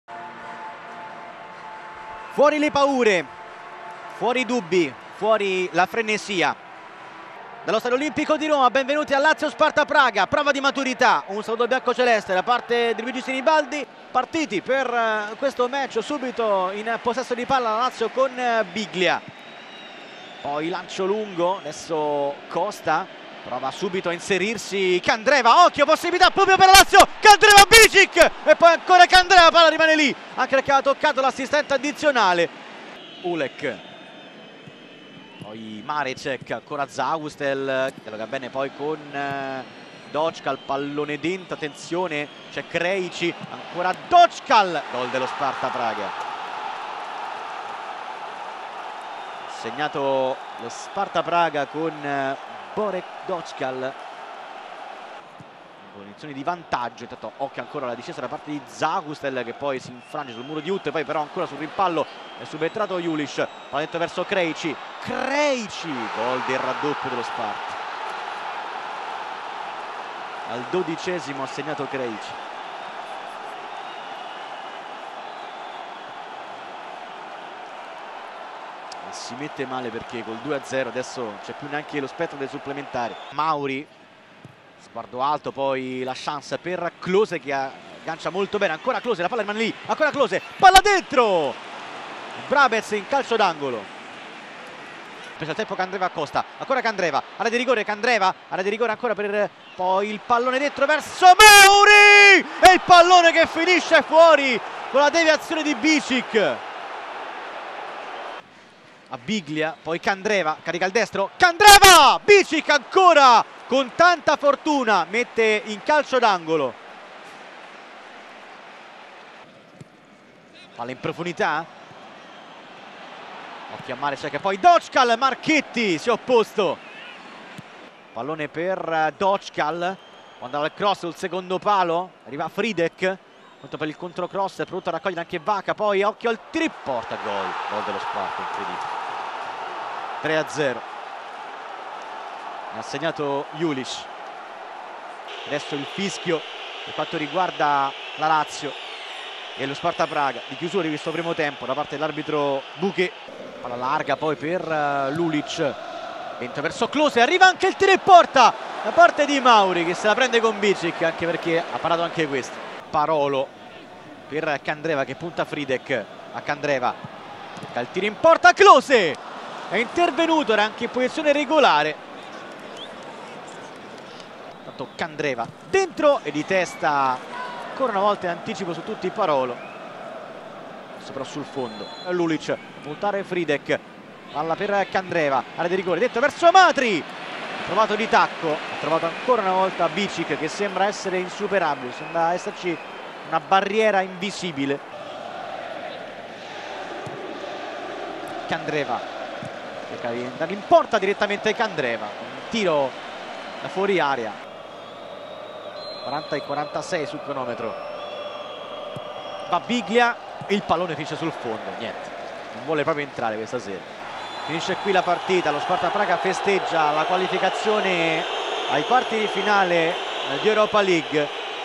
UEL | Highlights Lazio-Sparta Praga 0-3
Gli highlights degli Ottavi di Finale di UEFA Europa League, gara di ritorno, Lazio-Sparta Praga 0-3 allo Stadio Olimpico di Roma, tratti da Lazio Style Channel, 233 Sky.